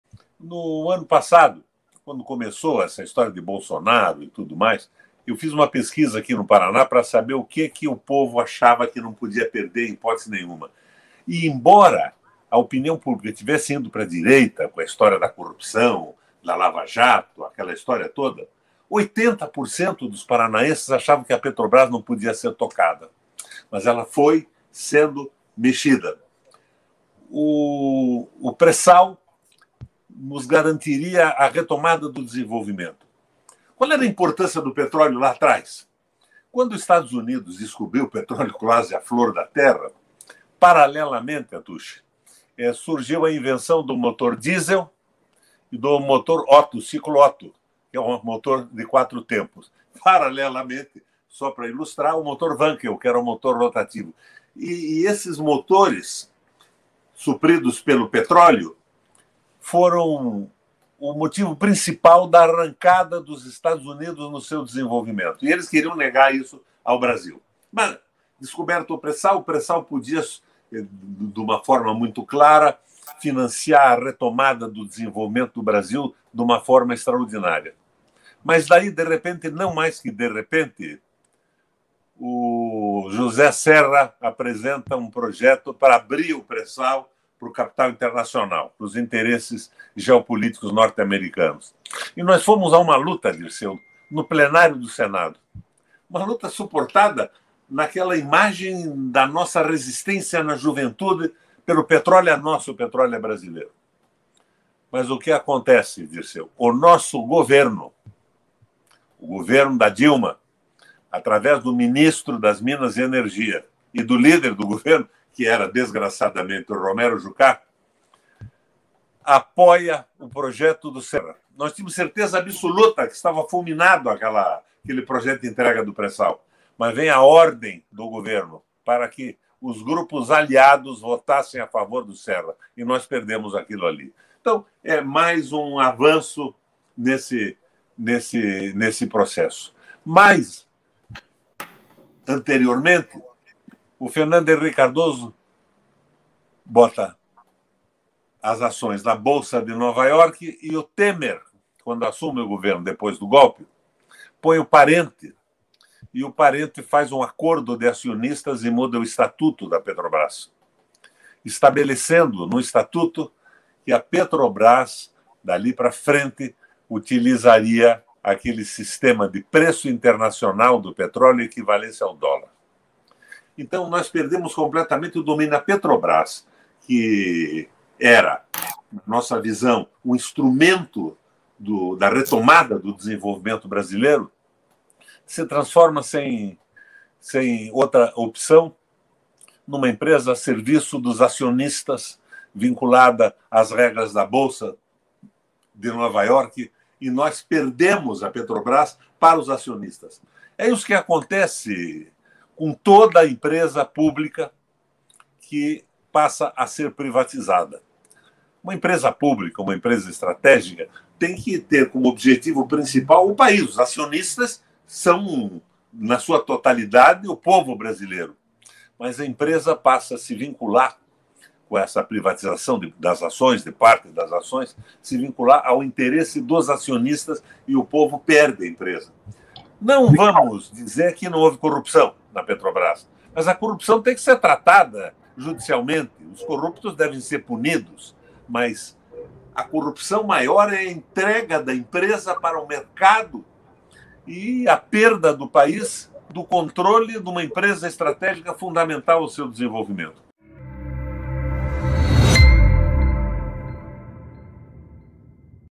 PERDA DA PETROBRÁS COMEÇOU QUANDO FHC COLOCOU AÇÕES EM NOVA YORK | Cortes 247
Roberto Requião (MDB) é ex-senador, ex-prefeito de Curitiba e ex-governador do Paraná.